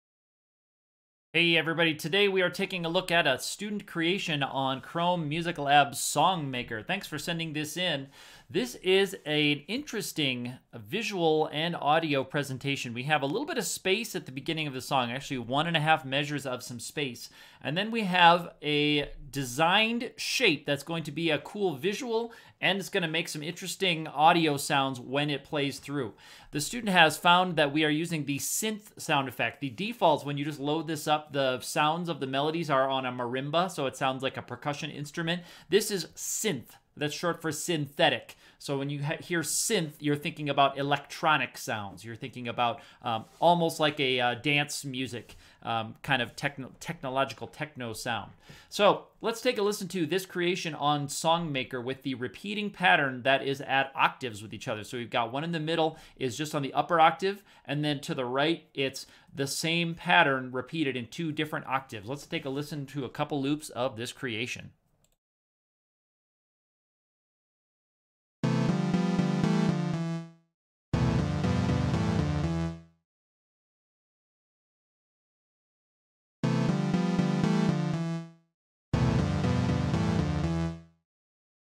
Superhero Student Creation
Using Chrome Music Lab Song Maker.